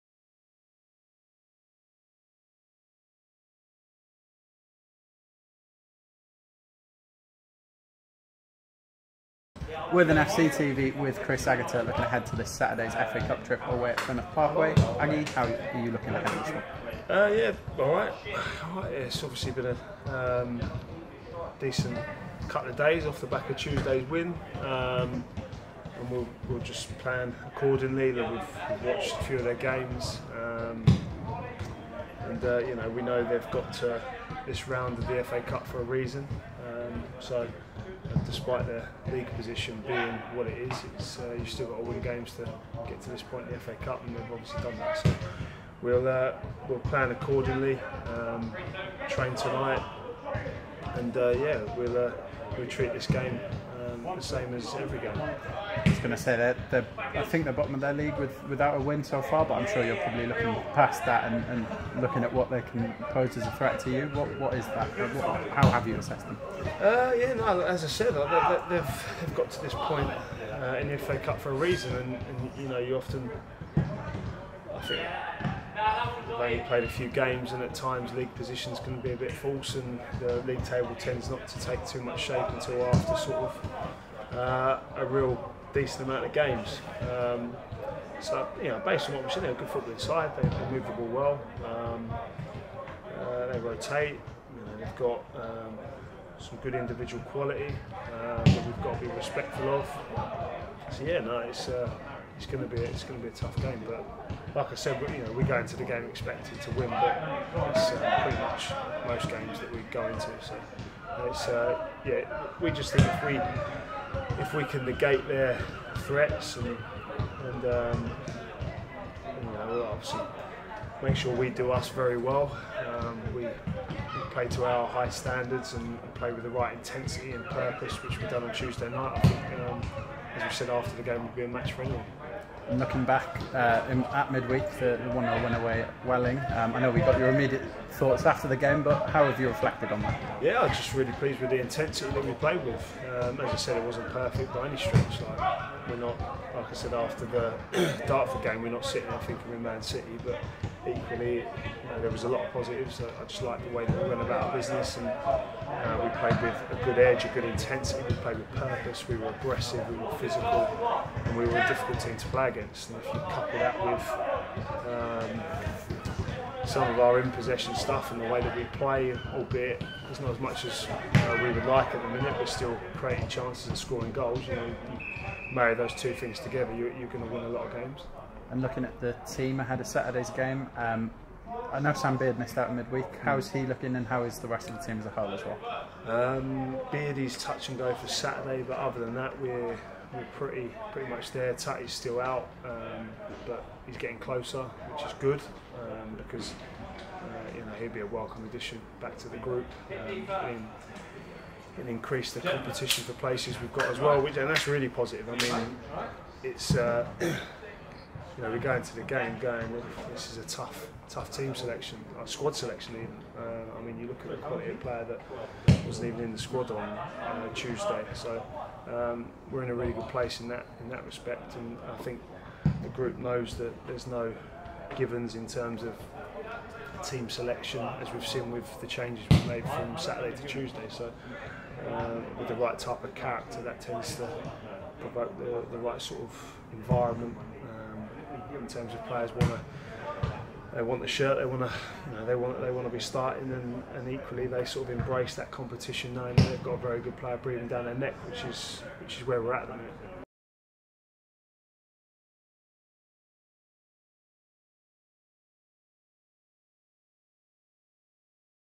"Happy with the intensity we played with on Tuesday" | Chris Agutter | Plymouth Parkway Preview
Chris Agutter looks ahead towards Saturday's FA Cup fixture against Plymouth Parkway.